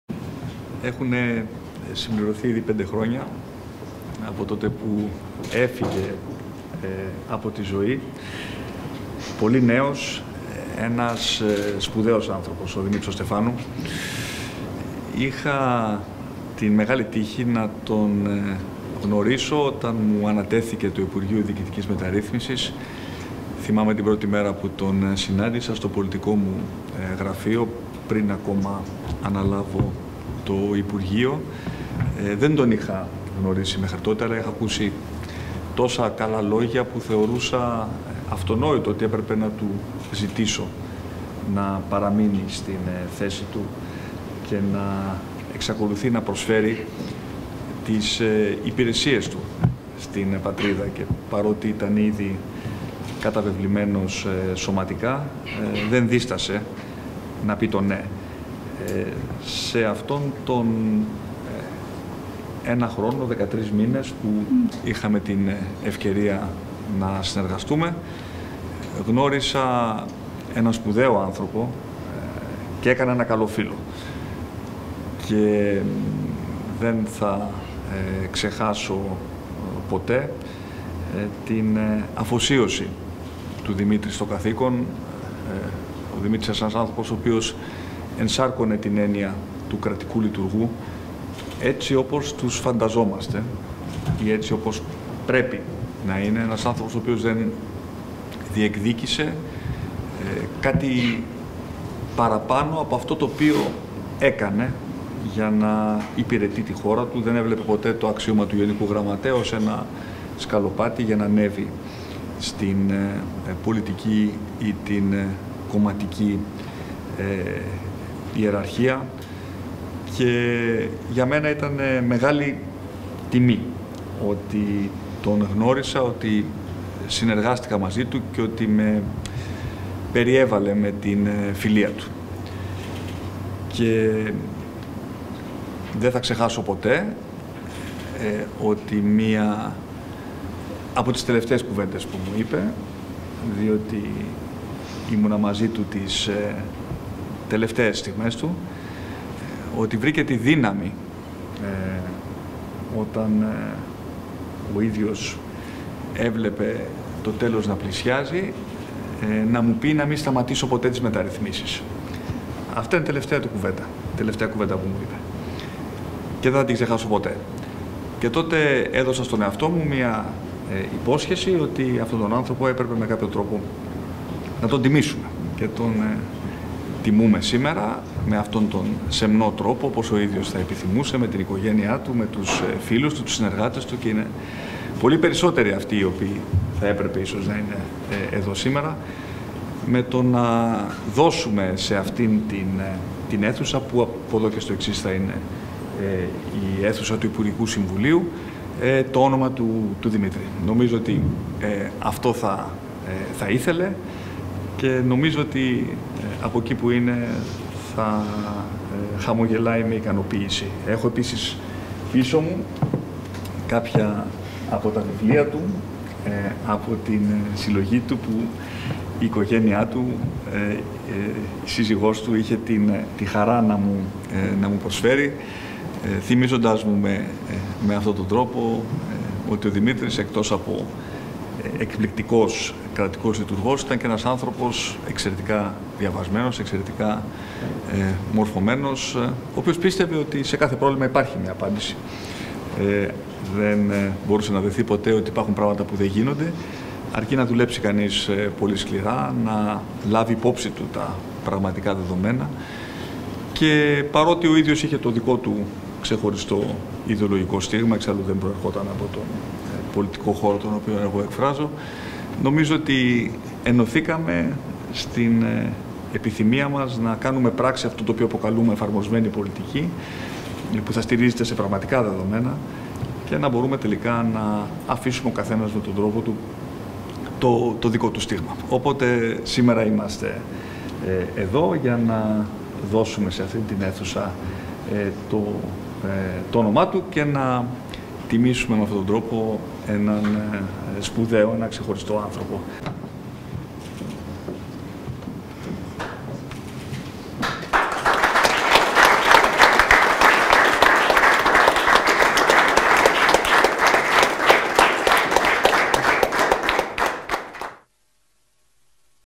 Ομιλία του Πρωθυπουργού Κ. Μητσοτάκη στην τελετή ονοματοδοσίας της αίθουσας «Δημήτρης Στεφάνου»
Τιμούμε σήμερα τον Δημήτρη Στεφάνου, έναν σπουδαίο άνθρωπο και έναν κρατικό λειτουργό που τον χαρακτήριζε απόλυτα η αίσθηση του καθήκοντος. Τελετή ονοματοδοσίας της αίθουσας συνεδριάσεων του υπουργικού συμβουλίου «Δημήτρης Στεφάνου» στο Μέγαρο Μαξίμου.